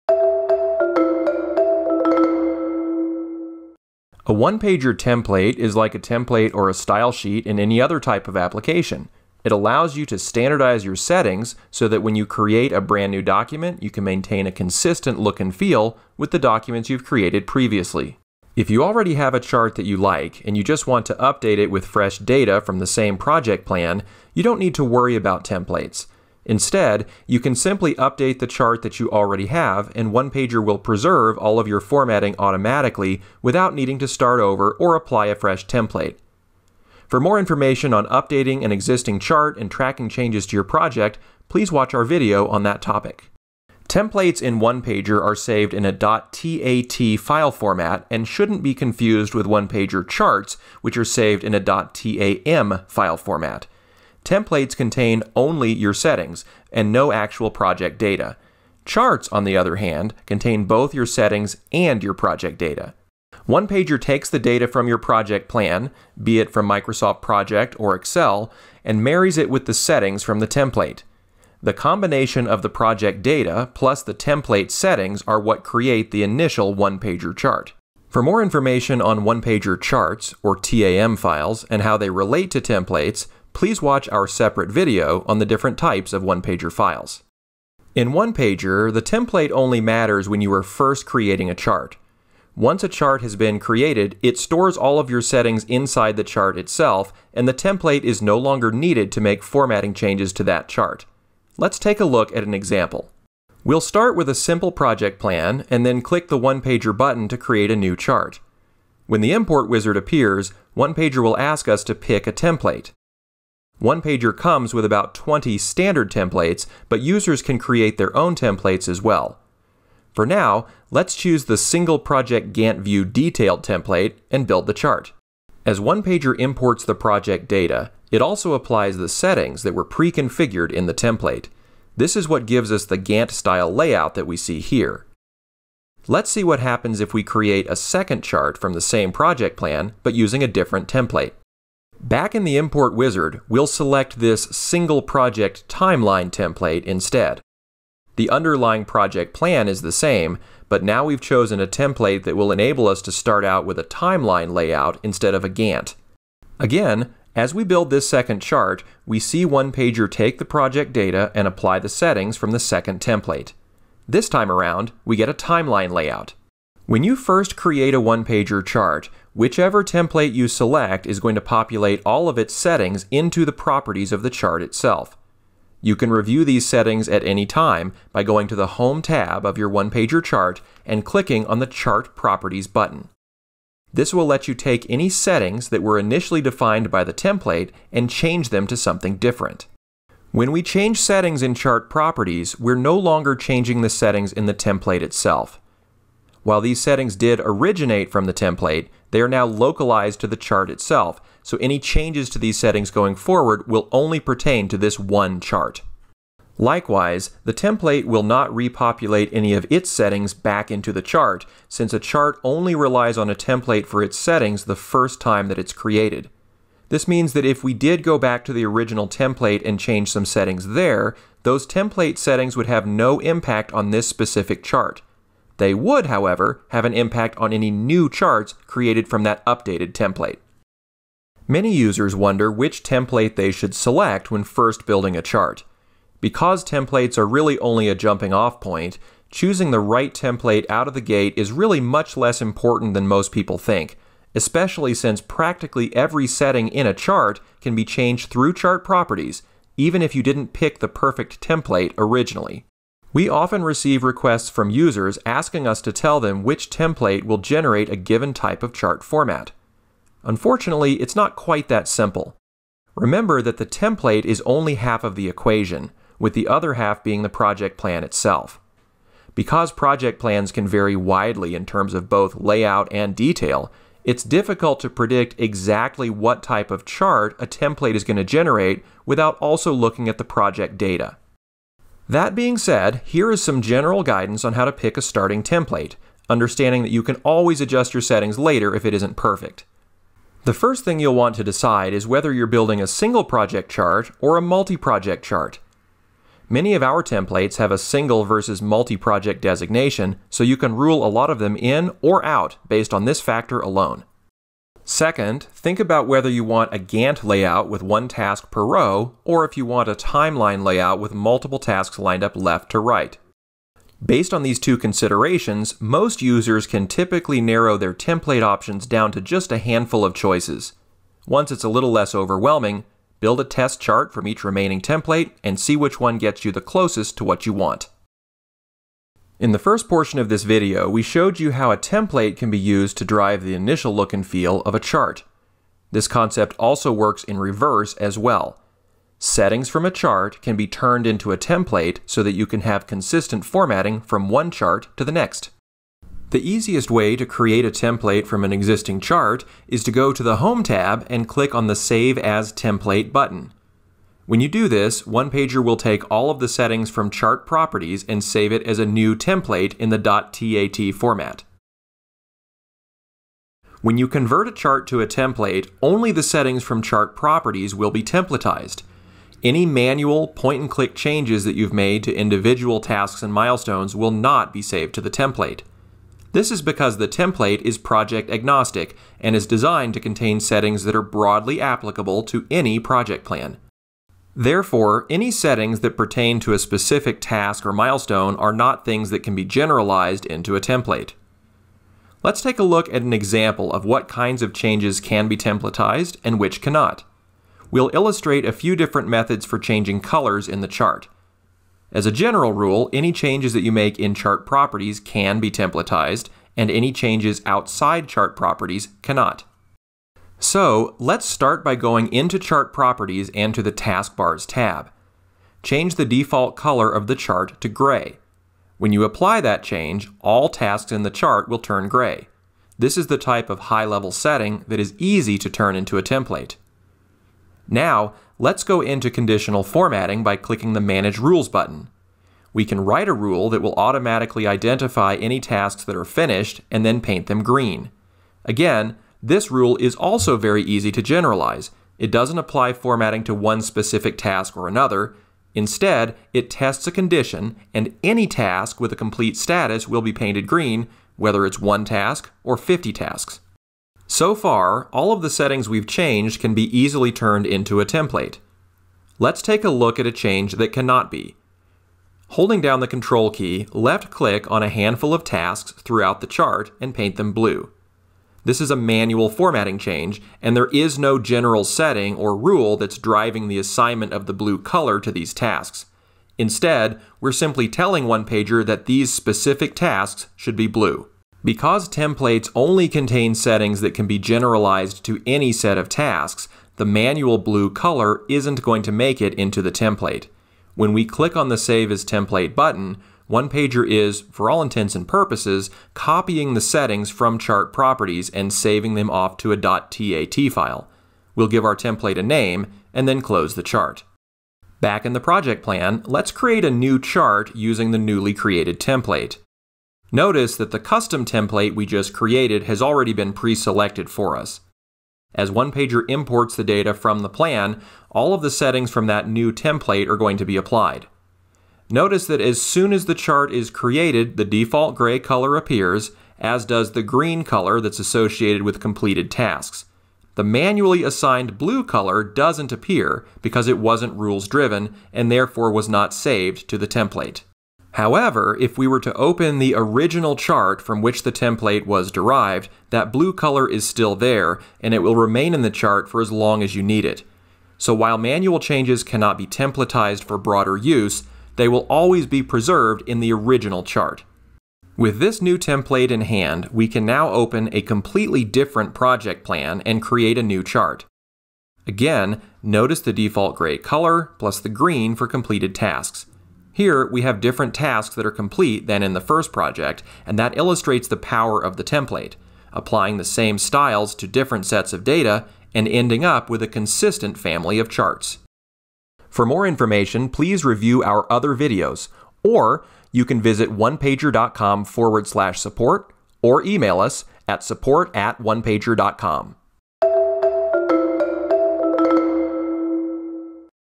OnePager Templates
Learn how to standardize your project timelines and Gantt charts in OnePager with sharable templates for your entire team.

- How to create a new chart from a template
- How to adjust settings in your chart to deviate from the template
- How to pick the best template for your needs
- How to create a template from a chart that you already have
- What settings can and cannot be included in a template